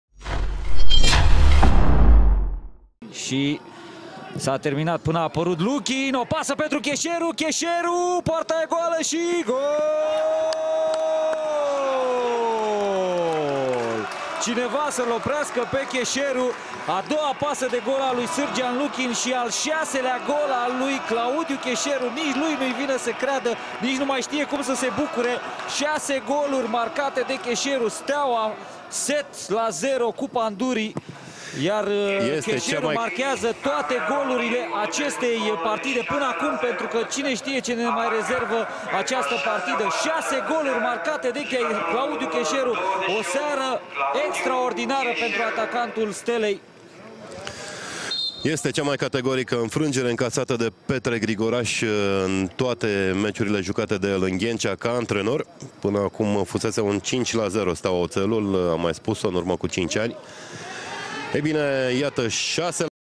Ce nebunie de meci! Keşeru, one-man show în meciul cu Pandurii
Claudiu Keşeru a marcat al şaselea gol în partida cu Pandurii, în minutul 86.